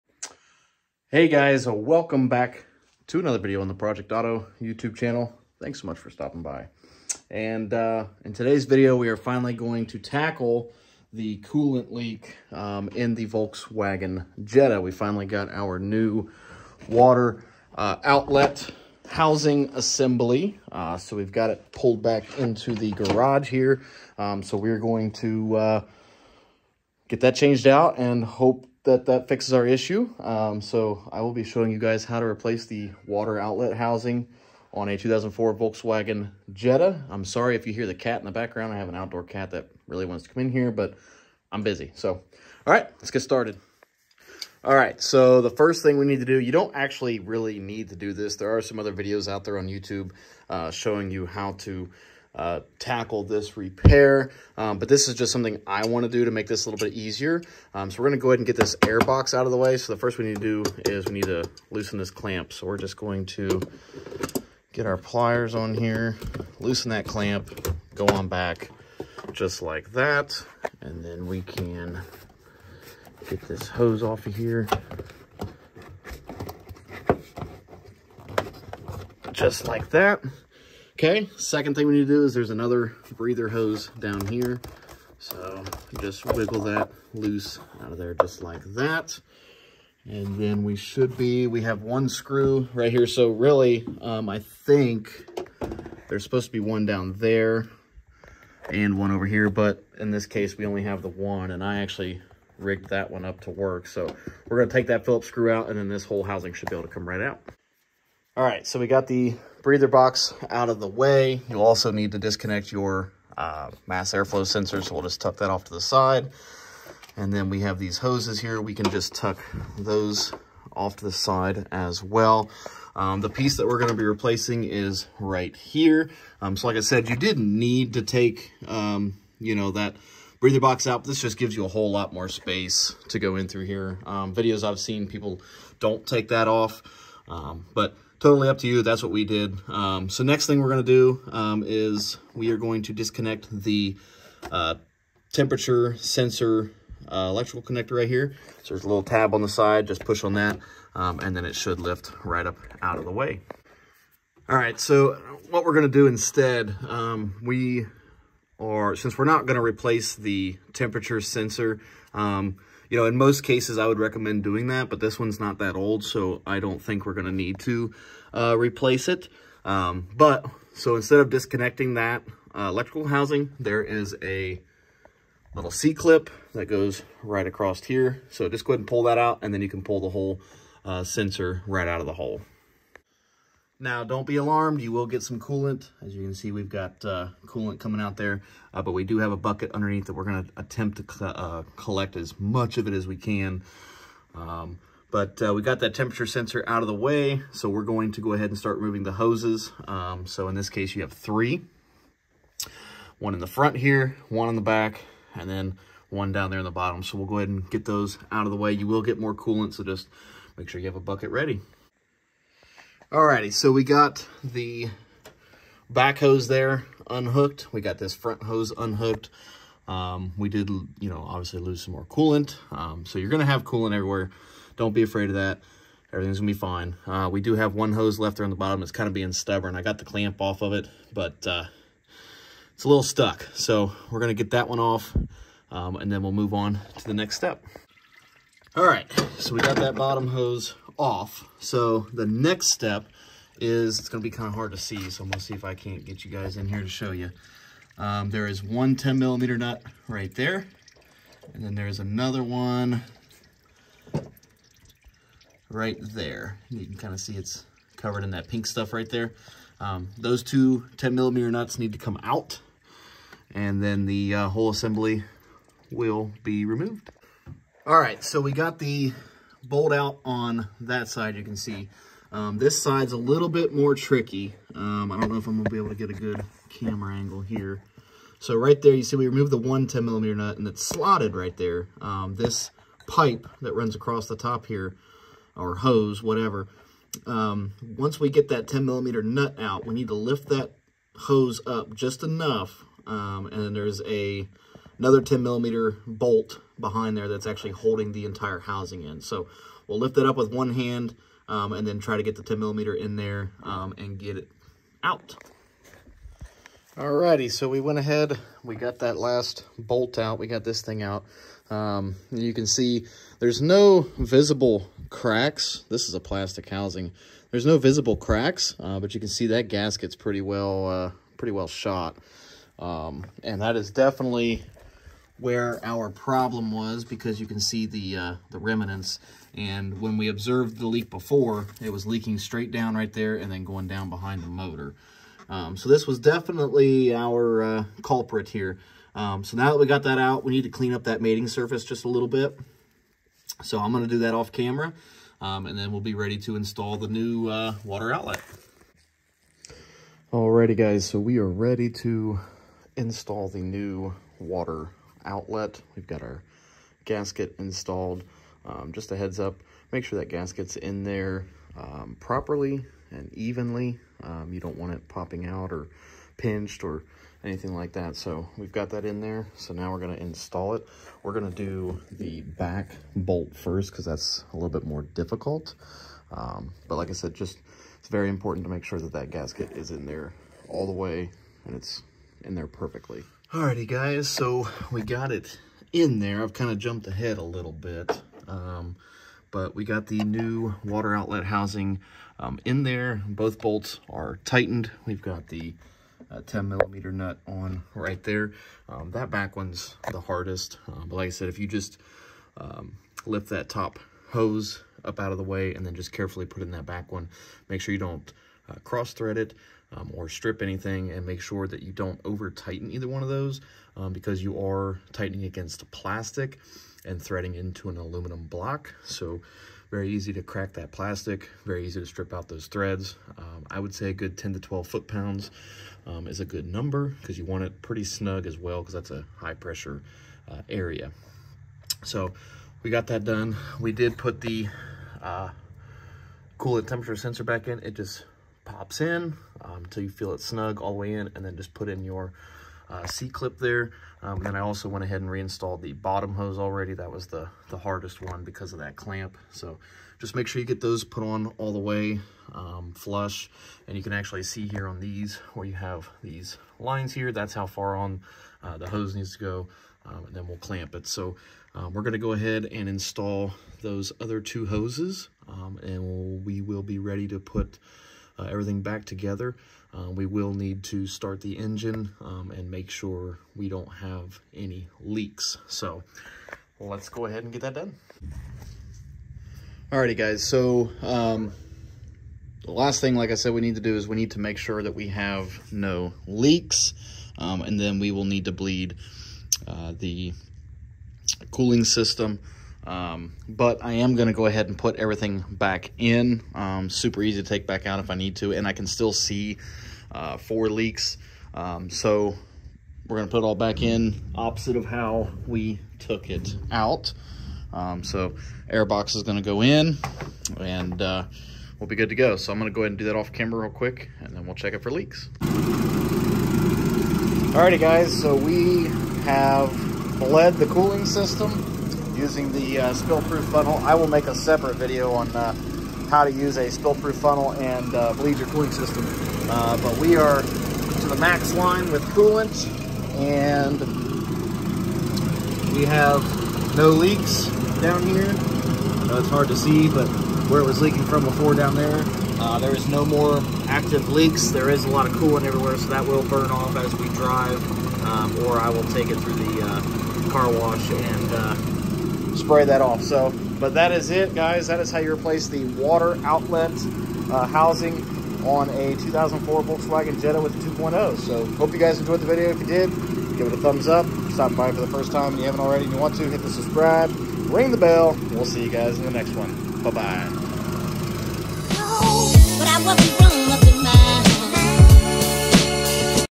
How to | Change the Water Outlet housing on a 2004 VW Jetta 2.0
Today we gotta tackle this coolant leak on the Jetta. How to change out the water outlet housing….enjoy